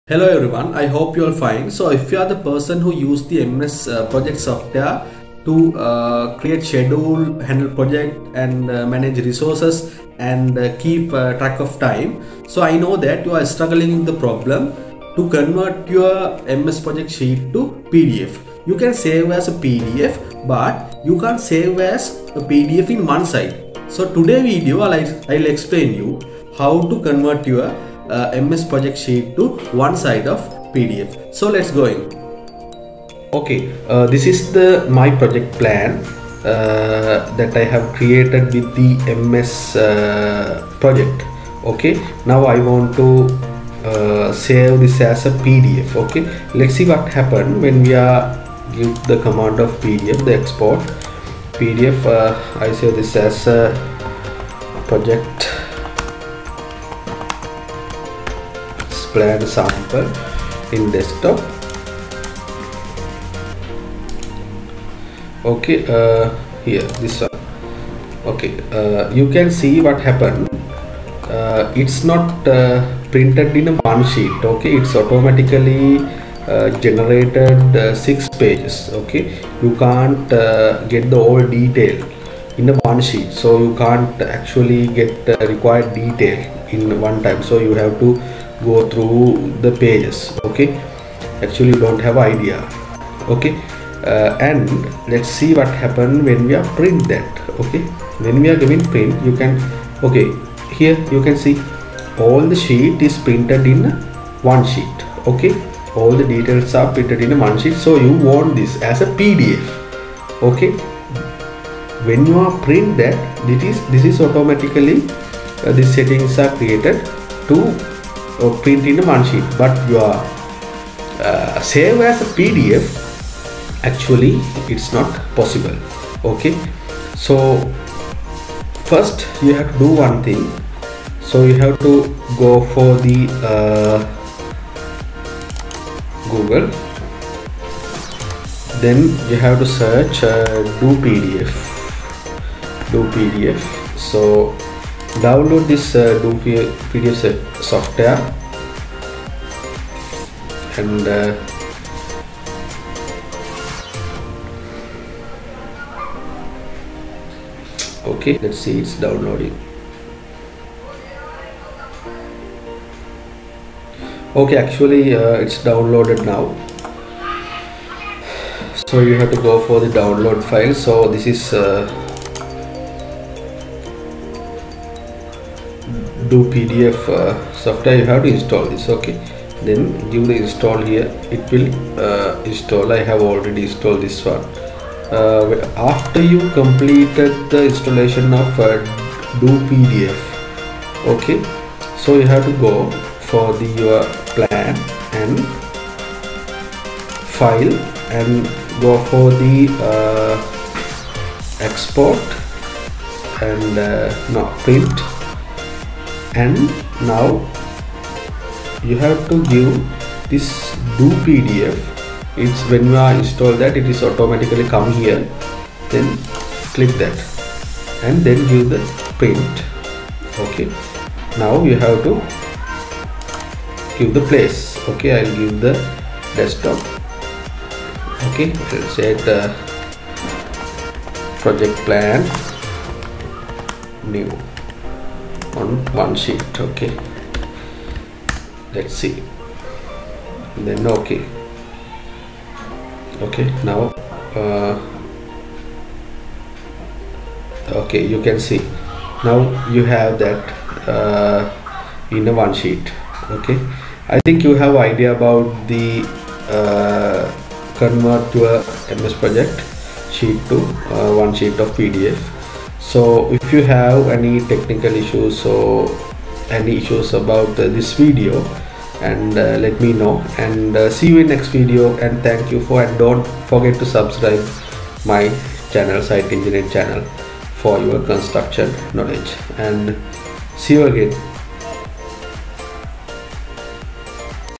MS project|Microsoft project pdf|how to create pdf in ms project|convert ms project to pdf one sheet
This video help you to create in pdf in Microsoft project